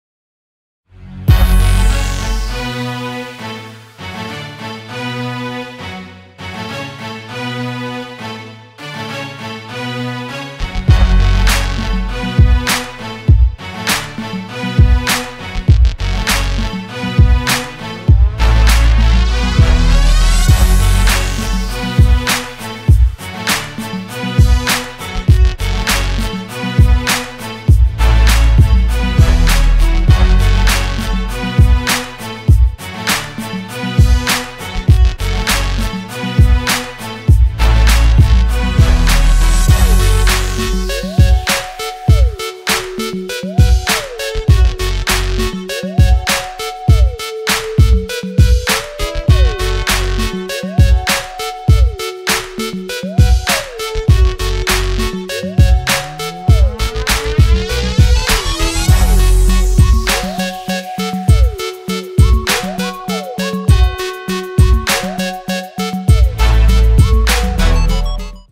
Warren Thompson Ultimate Highlights
Highlights of Armwood's (FL) 4-Star  Wide Receiver Warren Thompson.